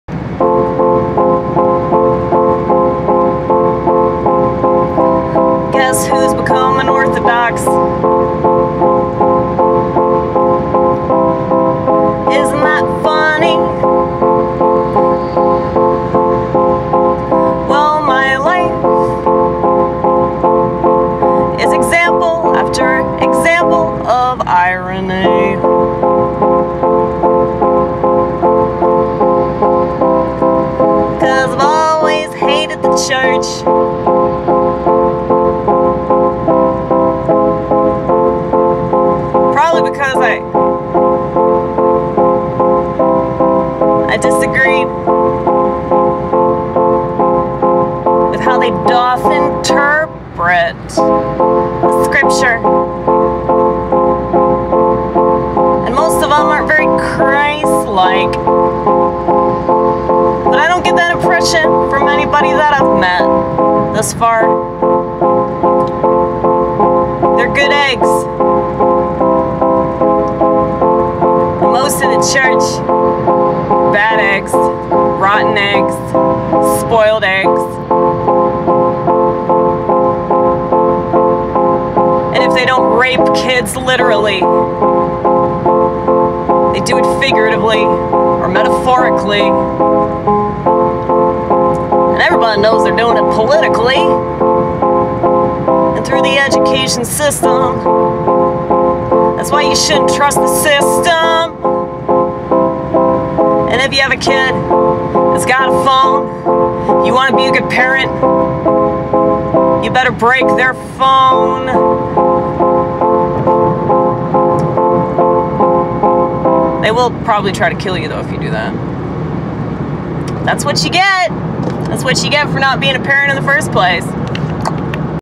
my life: an ongoing example of irony
guess who's becoming orthodox
always hates the church
most church people ain't Christ like
they're good eggs from what i've seen
bad, rotten, spoiled eggs everywhere else
that's what chu get for not being a parent in the first place